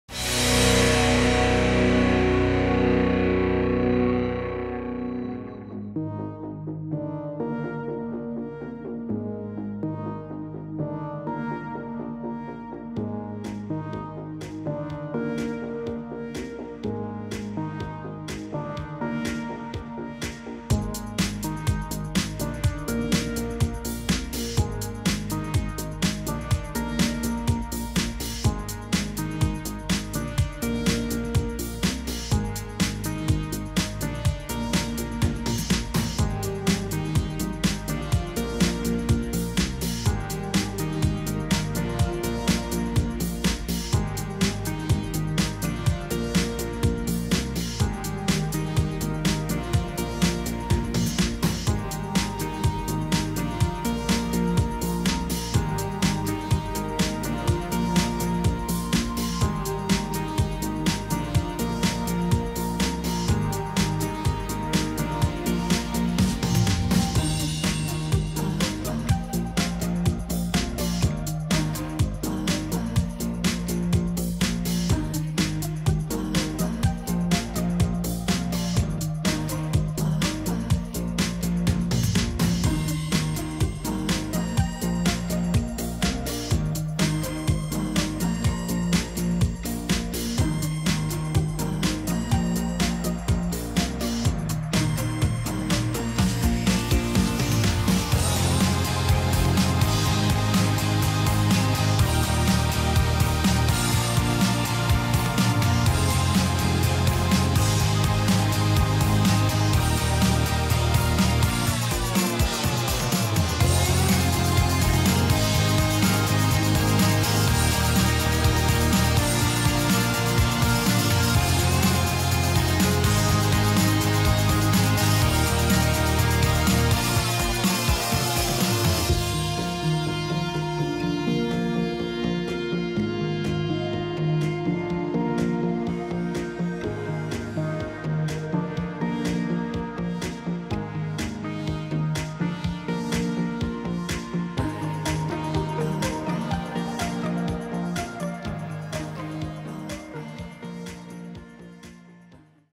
Resumen: CP Cacereño - Getafe CF B (Segunda Federación Gr.V 23/24)
Imágenes del partido entre el CP Cacereño y el Getafe CF B, desde el estadio 'Príncipe Felipe' de Cáceres.